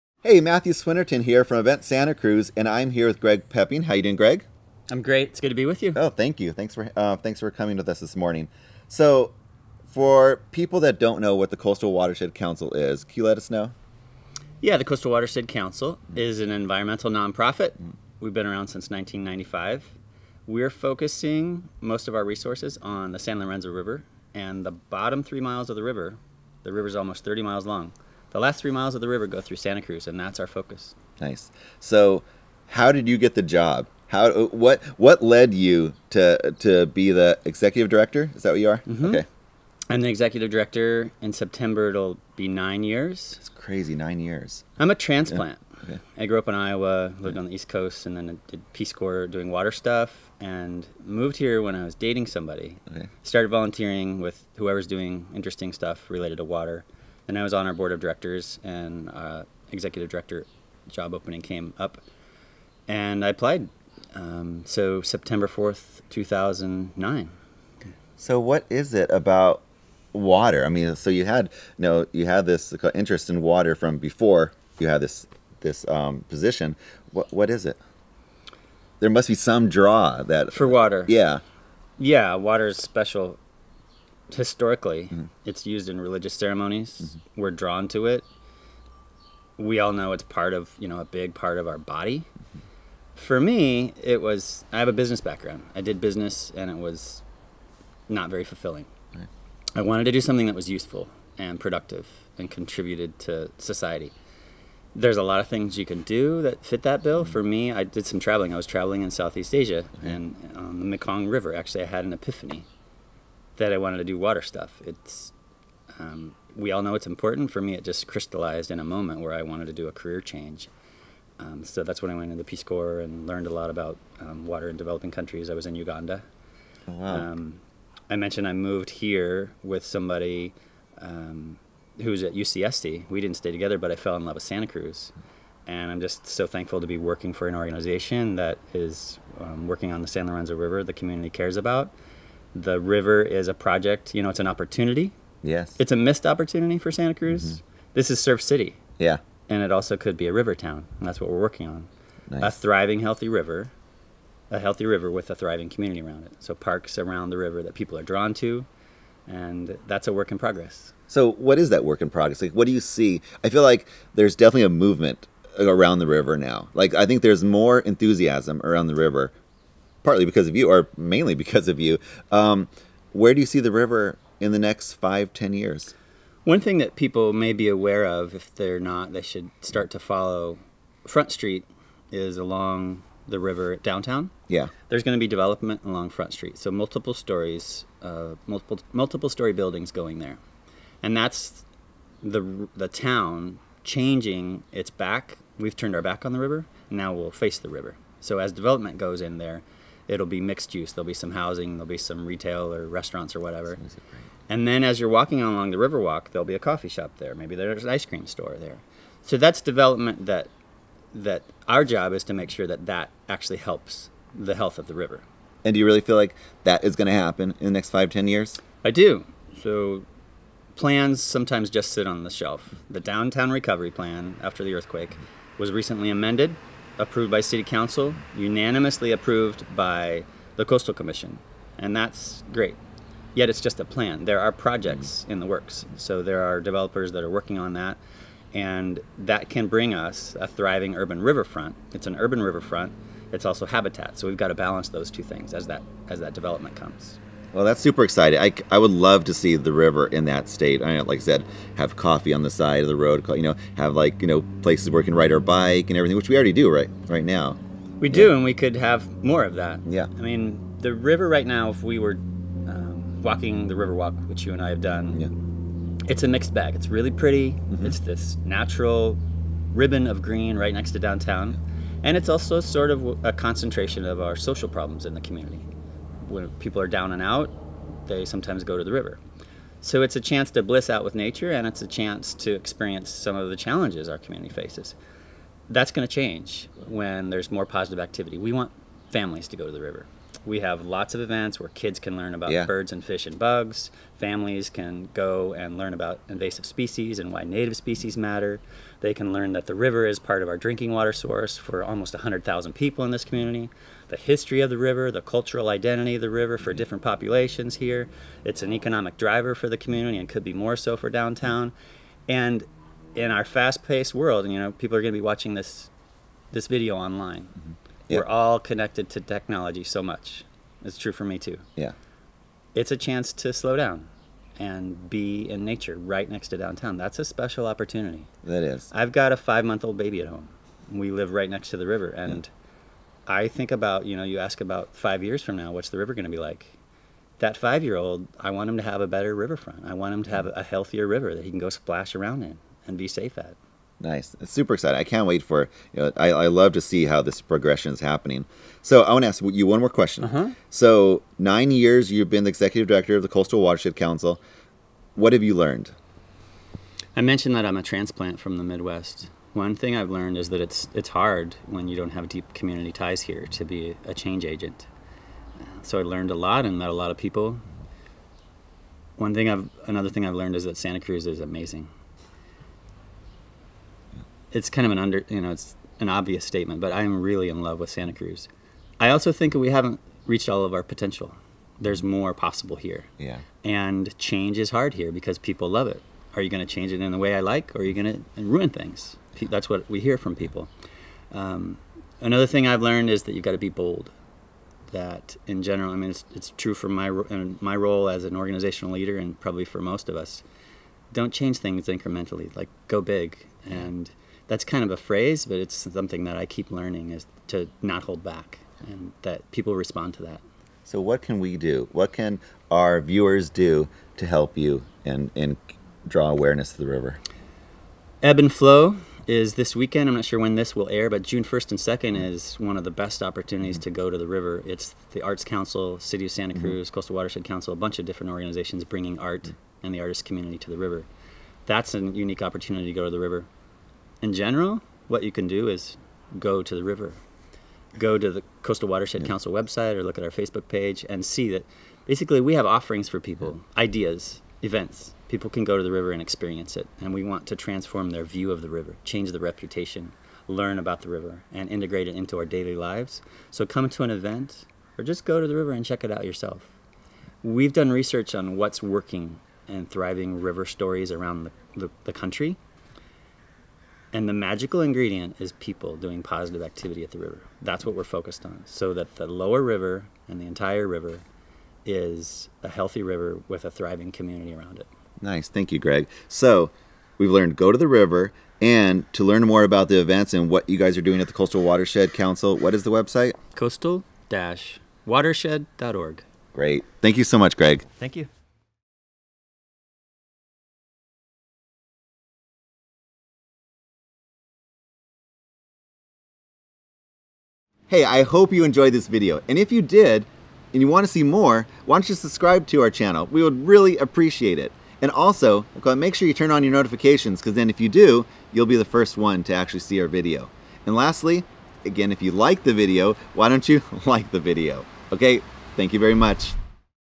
Is Santa Cruz a River Town? Interview with Greg Pepping
Is Santa Cruz a River Town? Check out our interview with Greg Pepping, the Executive Director of the Coastal Watershed Council and hear what he has to say.

Learn more about what they do and how you can get involved

Generating media for impact, from consulting to production to management.